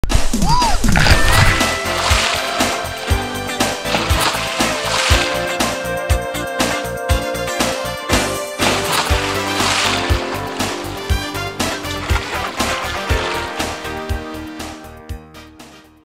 כייף בבריכות של המלון קלאב הוטל אילת
פארק מים אקזוטי ענק!  5 בריכות שחיה למבוגרים ומשחק לילדים. מסלעה מרהיבה במרכז הפארק מוקפת בצמחיה, מעברים, גשרים, וצוללת מיוחדת במינה.
משטחי צל, שיזוף כיסאות ומיטות שיזוף.